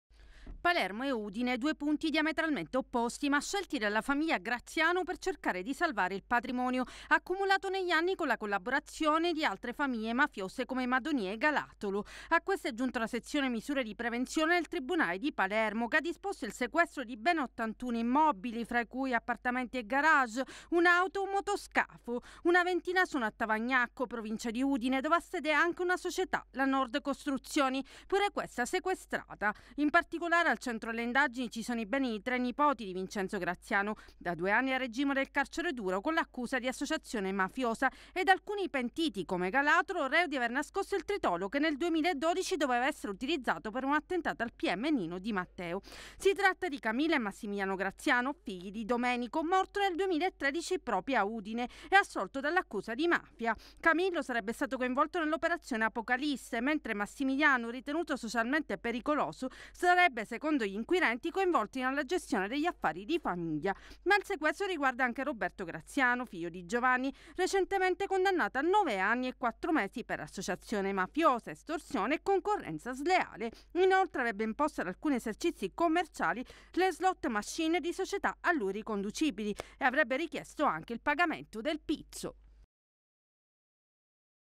Mafia, sigilli ai beni degli eredi Graziano [TgMed 19/05/2016]
Un maxi sequestro ha colpito i beni degli eredi
della famiglia dei Graziano. I sigilli sono
scattati per 81 immobili e 4 società dislocate tra
Palermo e Udine dove da tempo erano stati
spostati gli interessi economici della famiglia
mafiosa dell'Acquasanta.
AMBRA DRAGO